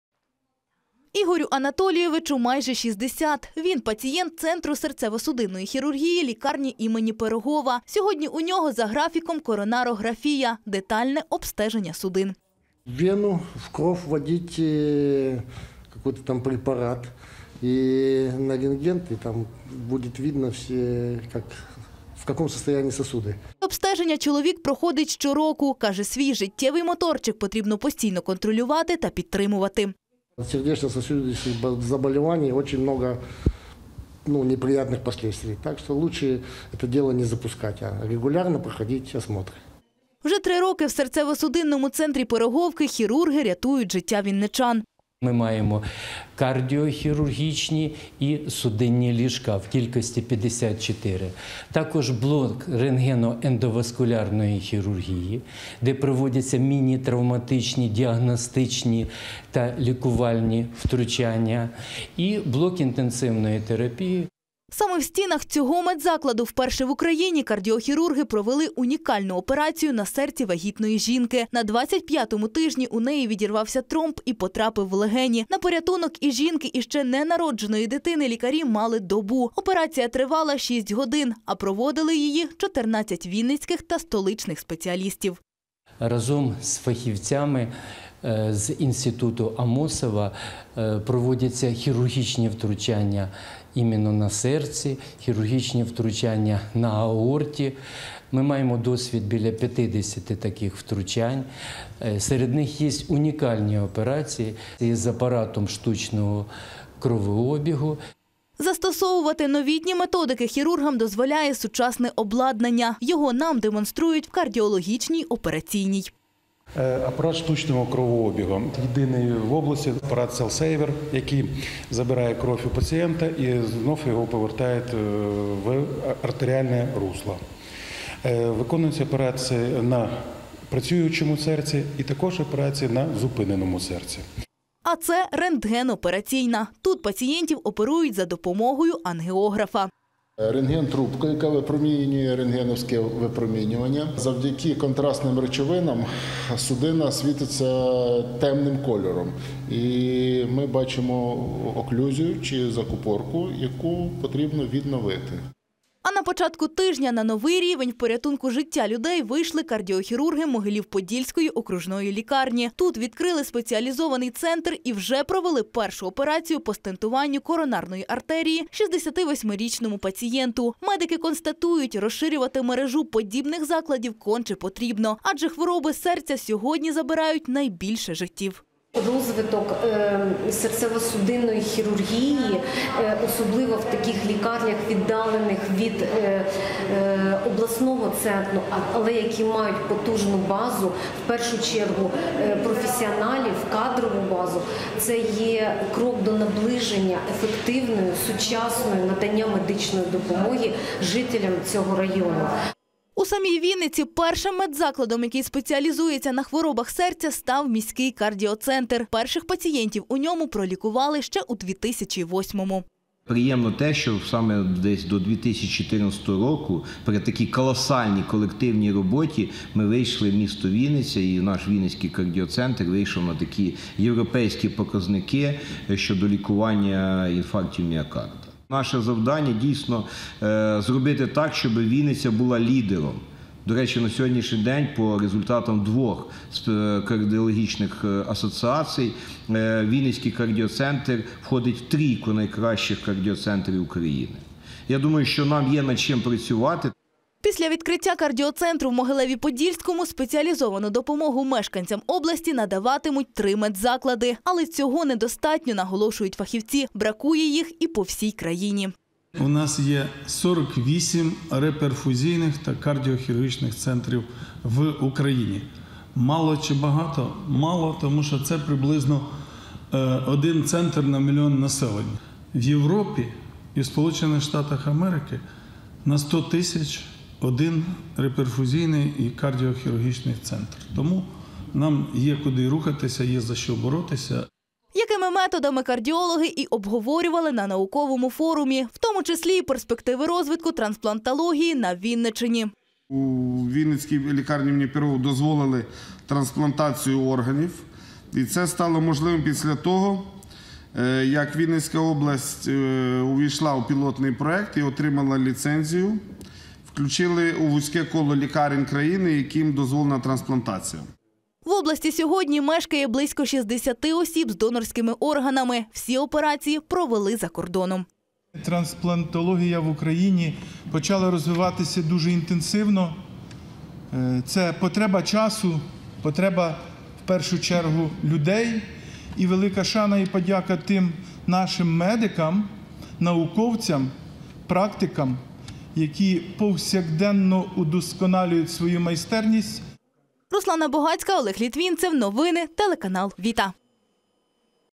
У Вінниці відкриють центр трансплантології
Вінницька медицина виходить на якісно новий рівень. В обласній лікарні ім. Пирогова готуються відкрити центр трансплантології, а в Могилеві Подільському вже працює спеціалізований кардіоцентр. Перспективи і сучасні методики допомоги вінничанам, які страждають від серцево-судинних хвороб, обговорювали академіки, науковці та лікарі разом із керівниками регіону.